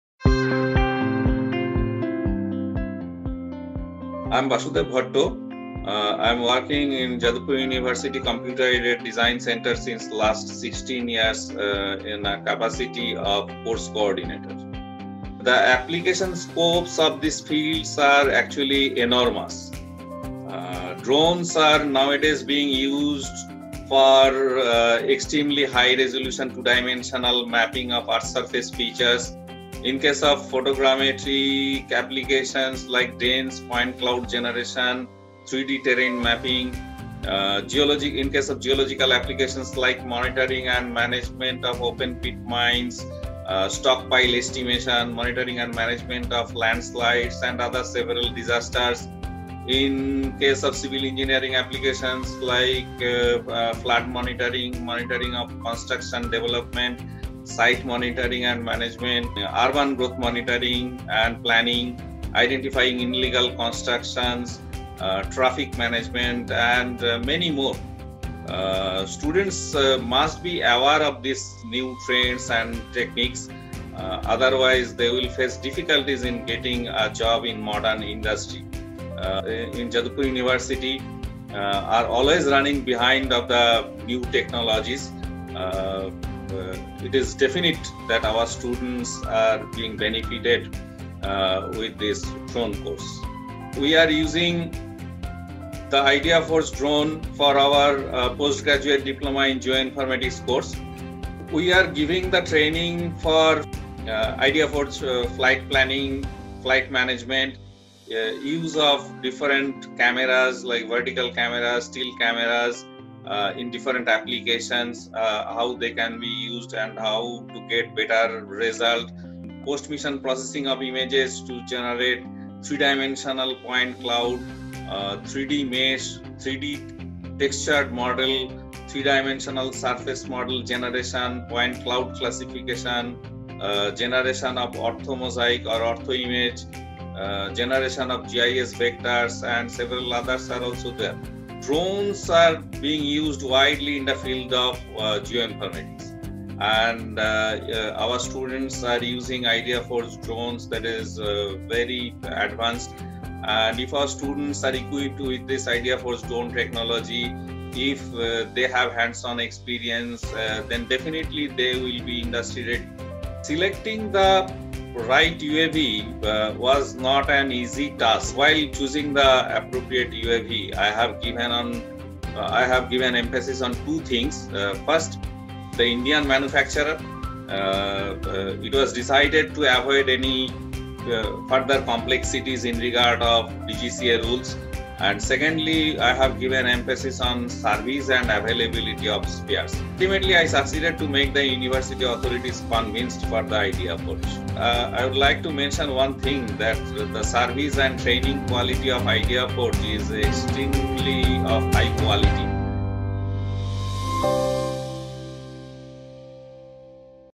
Drones in Education - Dr. Basudeb Bhatta, Jadavpur University is all praises for ideaForge drones
Dr. Basudeb Bhatta is Course Coordinator - Computer Aided Design Centre at Jadavpur University. He is using ideaForge NINJA UAV for providing practical training to students as a part of the Postgraduate Diploma in Geoinformatics course. Students are given training on flight planning, flight management, use of different cameras (vertical/tilt camera) in different applications, then post mission  processing of images to generate 3d point cloud, 3d mesh, 3d textured model, 3d surface model generation, point cloud classification, generation of orthomosaic,  generation of GIS vectors and several others.

He sees tremendous applications of drones in streams like Geo-informatics, Geography & Geology and Civil Engineering. According to him, drones are being used widely in the field of geoinformatics and if students are equipped with this technology and they have hands on experience then definitely they will be industry ready. They are being appreciated in the campus interview by the recruiter.

He adds, "I want to mention that the service and training of ideaForge is of extremely high quality."